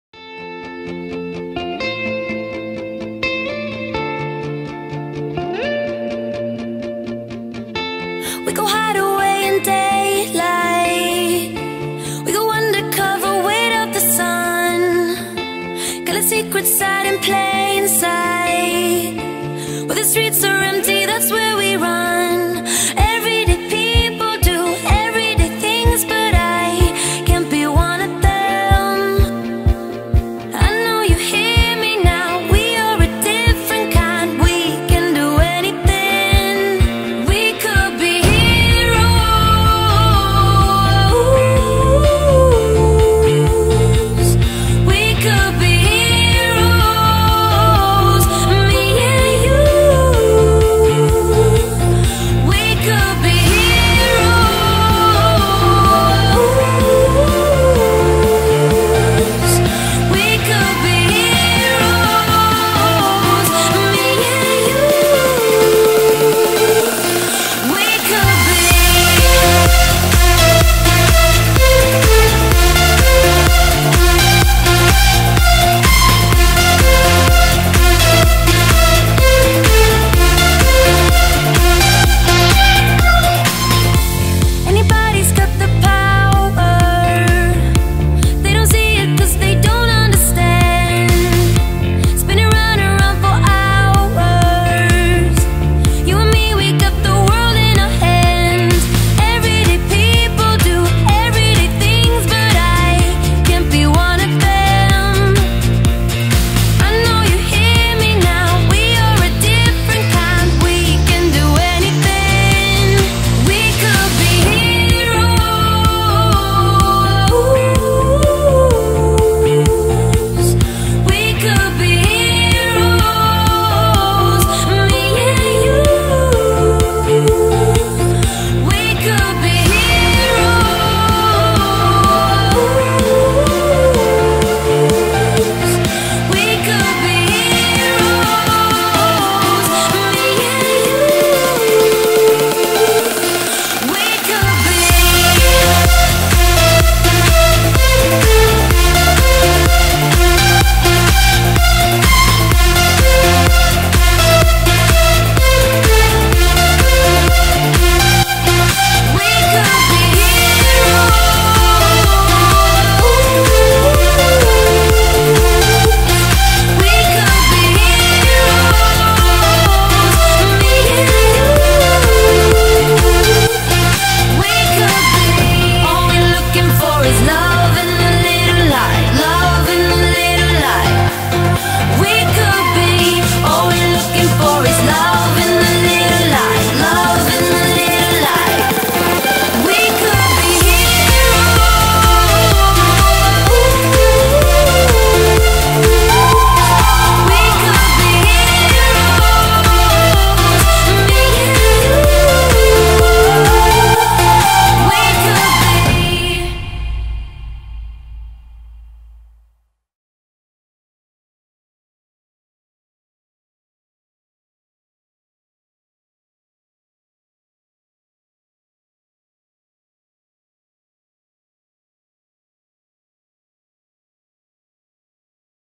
Xiaomi Mi 4K Drone - Carrickfergus Castle
Some cool arial views of the Norman castle in Carrickfergus, Northern Ireland.
Beautiful day.